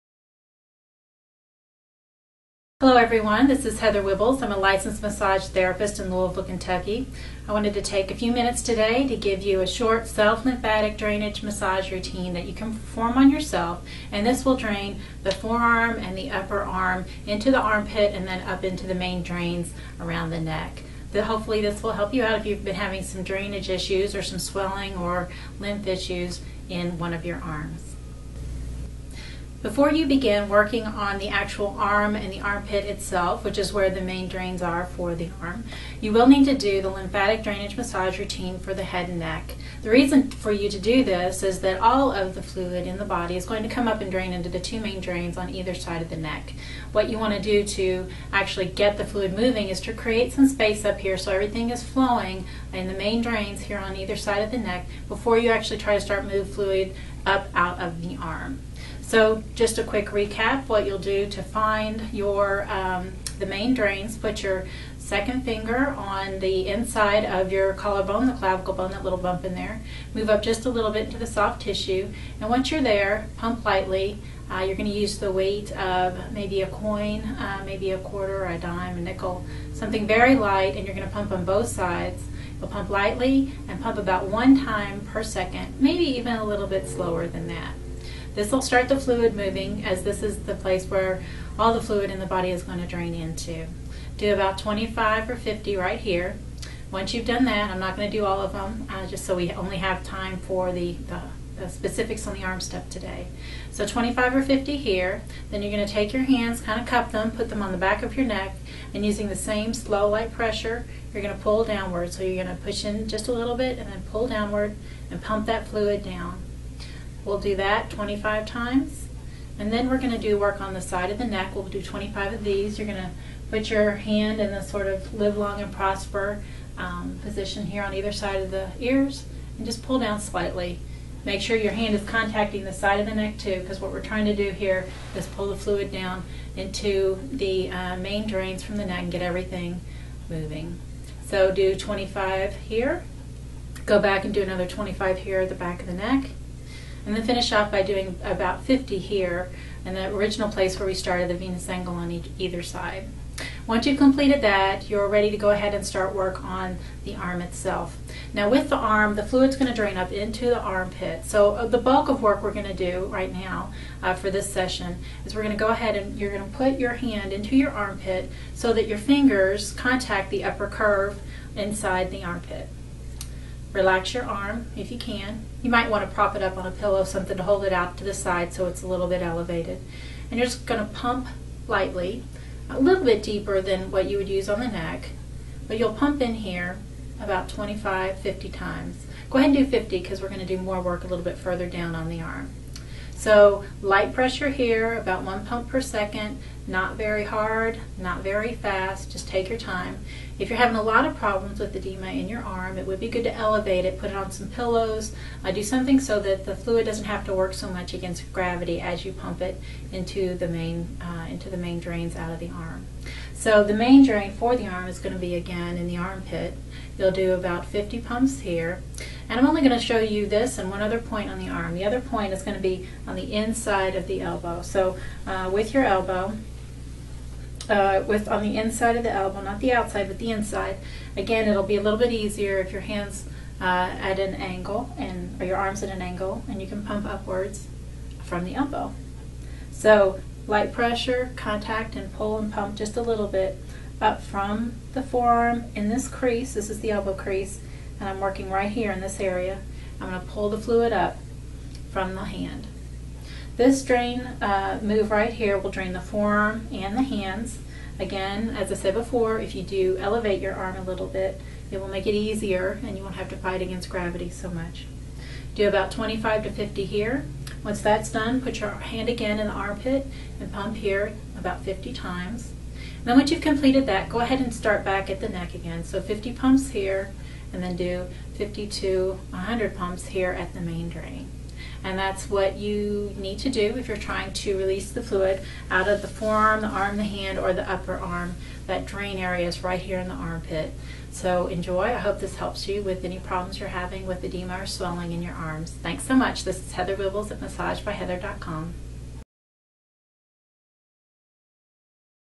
Self Lymphatic Drainage for the Arm from Louisville's MassageByHeather.com
- Learn an easy self-massage routine to drain fluid and reduce swelling from the arm. If you're a breast cancer survivor or have extreme edema in the arm, please consult your physician before performing this on yourself.

Before you work the arm, you'll need to clear the main drains at either side of the neck, so go to this video for a more in-depth instruction on clearing the head and neck

Contraindications include high risk pregnancy. If you're pregnant, consult your physician before performing this on yourself. Other contraindications for Lymphatic Drainage Massage work in general are as follows:

1. Fever, acute infection, early onset inflammatory disease. You need to wait until the acute phase is over and the fever is broken.
2. Circulatory system problems, especially thrombosis. If there is any risk of embolism of phlebitis, do not use LDM and see your physician immediately
3. Cardiac issues such as heart disease, acute angina pectoris or coronary thrombosis (heart attack). Using LDM increases the fluid load on the heart, and compromised cardiac systems should not be subjected to LDM.
4. Active bleeding, internal or external.
5. Active malignant cancers, undiagnosed lumps, or tumors whose origins have not been determined by your physician.
6. High risk pregnancy or late term pregnancy with complications